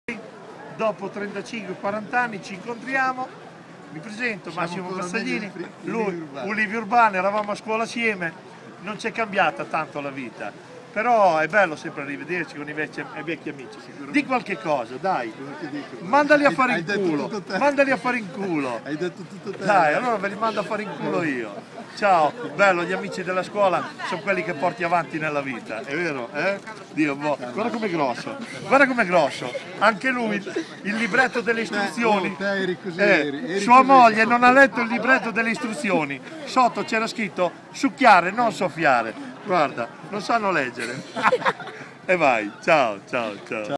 Dopo 40 anni rivedersi con i compagni di scuola
Incontro tra amici